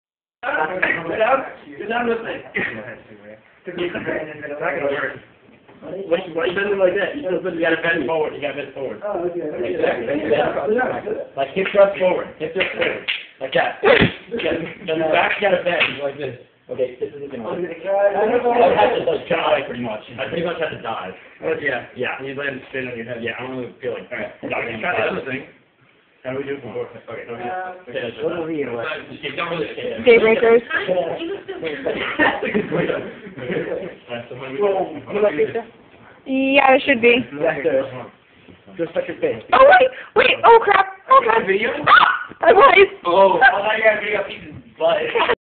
6/14/2010
They were being silly, and Peach picked me up and threw me over his shoulder. haha.